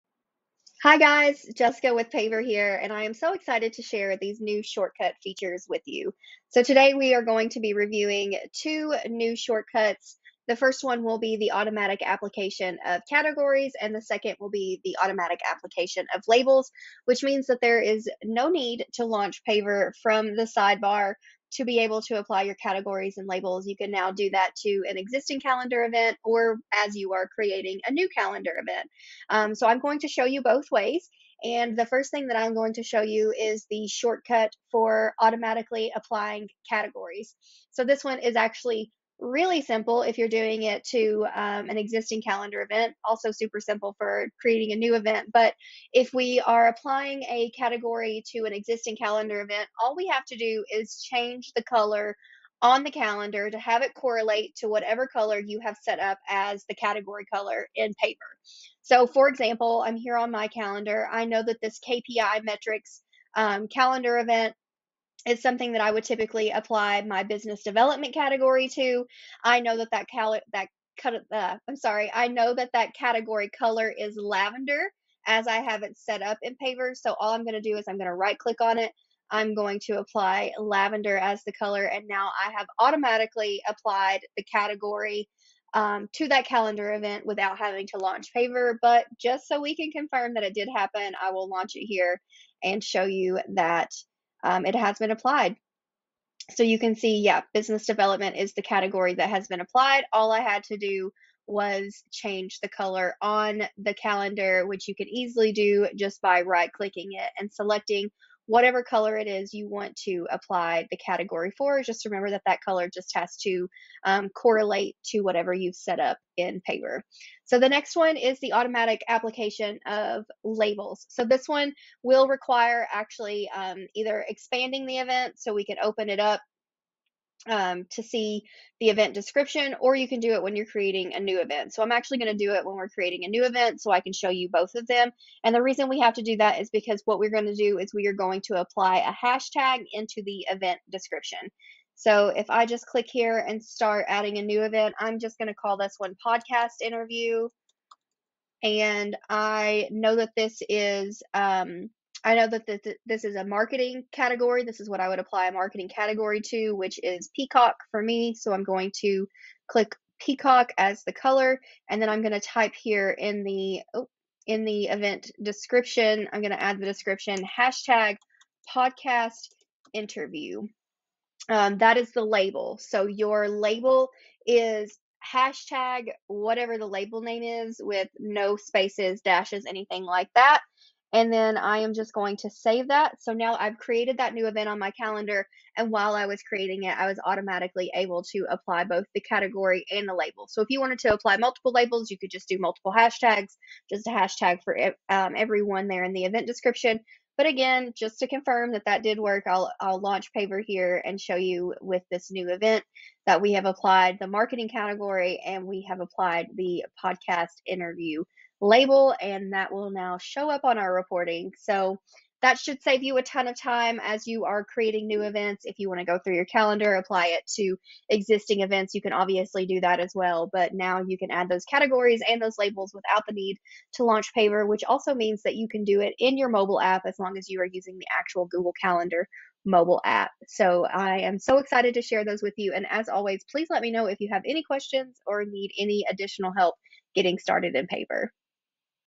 Add Categories & Labels to Google Calendar Events
New shortcuts for automatically applying categories & labels using pav*r.